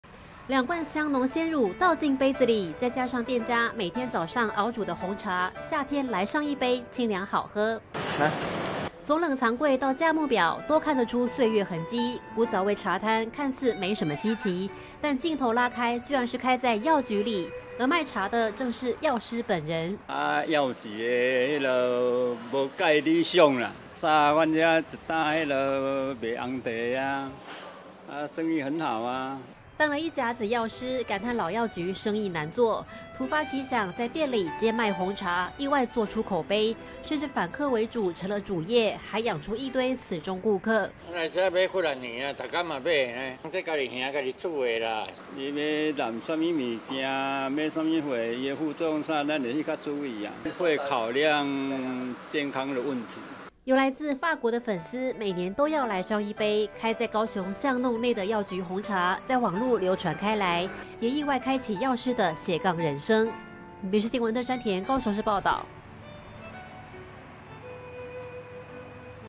感嘆藥局生意難做 藥師協槓「賣紅茶」意外爆紅－民視新聞
【民視即時新聞】賣飲料不稀奇，帶您來到高雄一家飲料攤，賣的是自家熬煮的紅茶、奶茶，但特別的是，飲料攤不但開在藥局裡，賣飲料的也是藥師本人。業者當了一甲子的藥師，感嘆老藥局生意難做，突發奇想在店裡兼賣紅茶，意外的做出口碑，甚至還有遠自法國的顧客，就愛這一味。